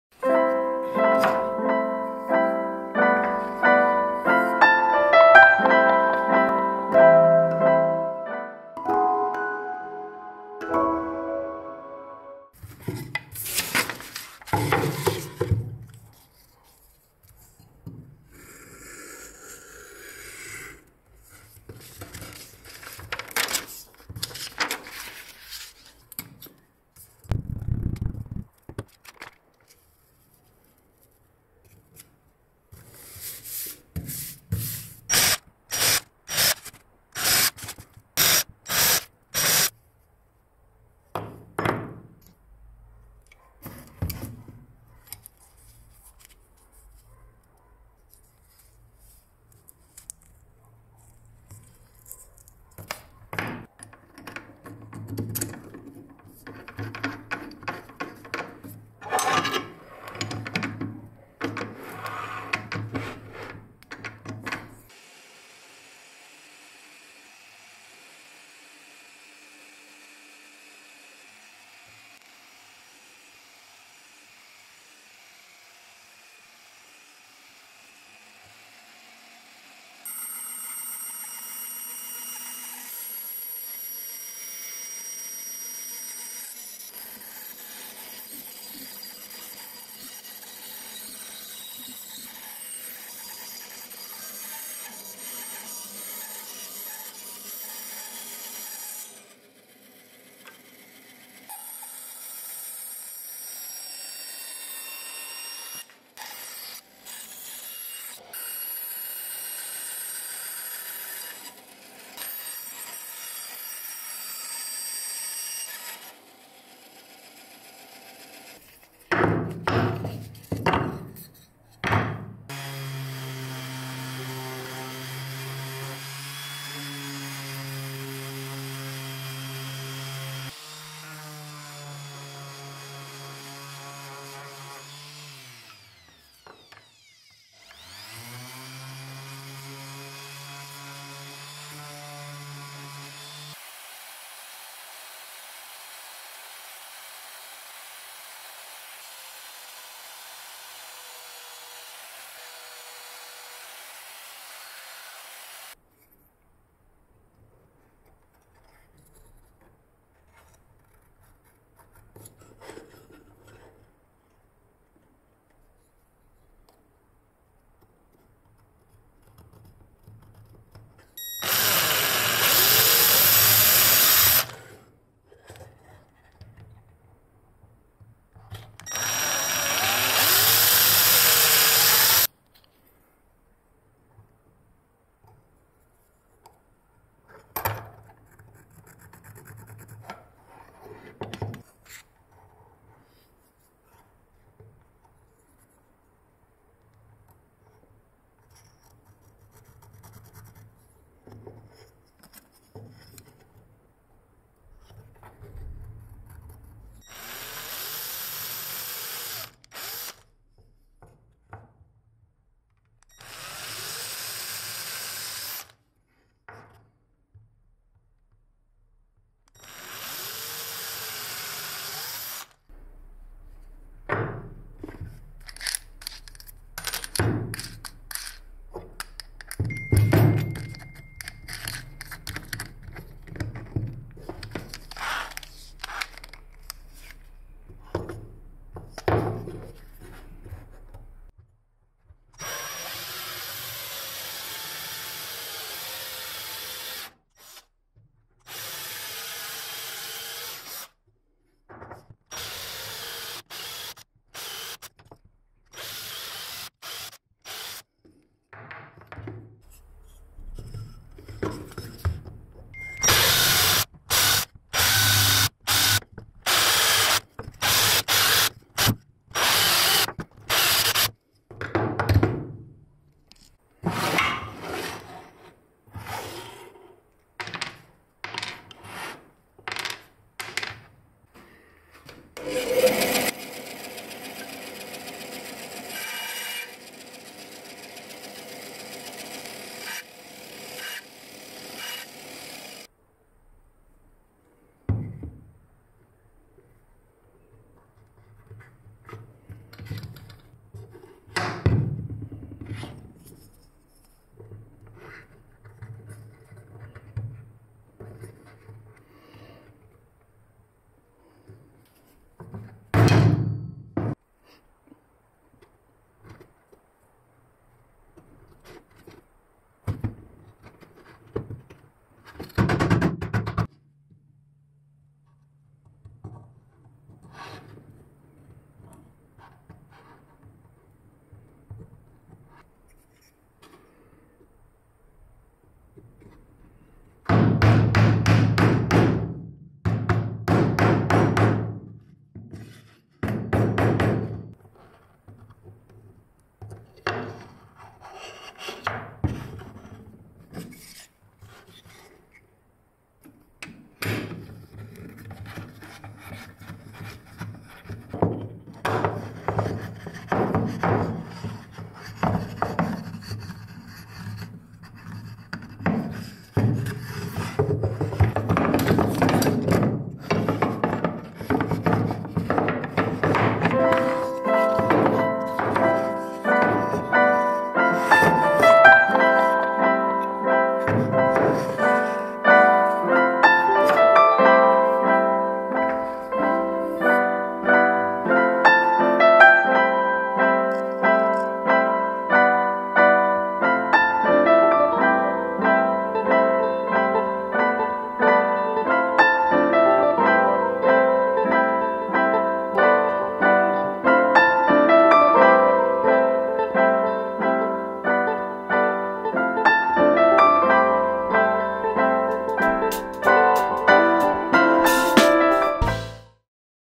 Hammer Rack. ( Tool Storage Wall French Cleat DIY)
Another video about organizing and storing the tool - "Hammer Rack".
I also design more complex ones for power tools. I hope this will be useful to someone.